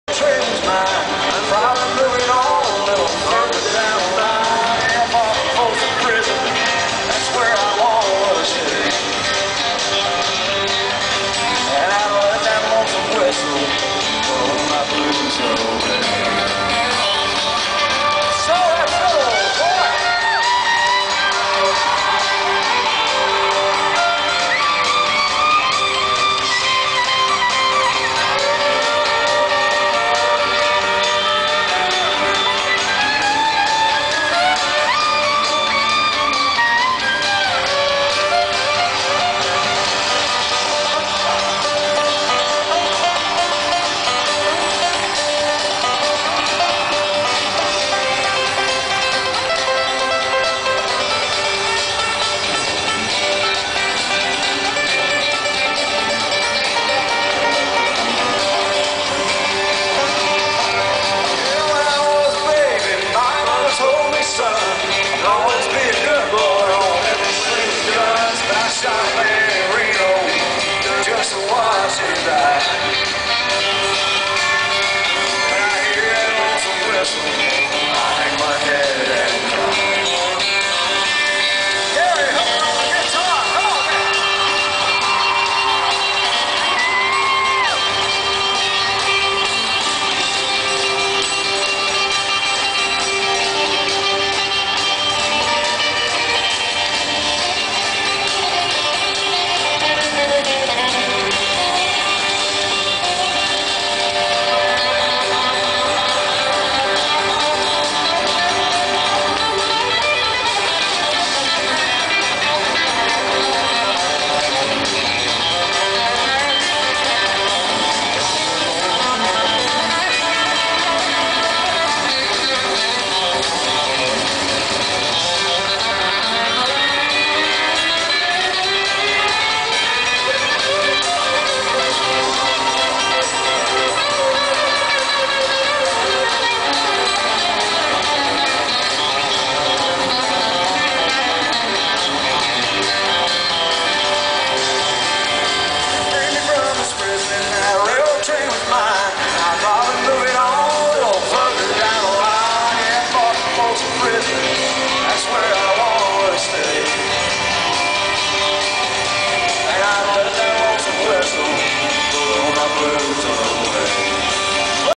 brad paisley concert 2007 in washinton state
brad redoes an old jonny cash sone and it kicks ass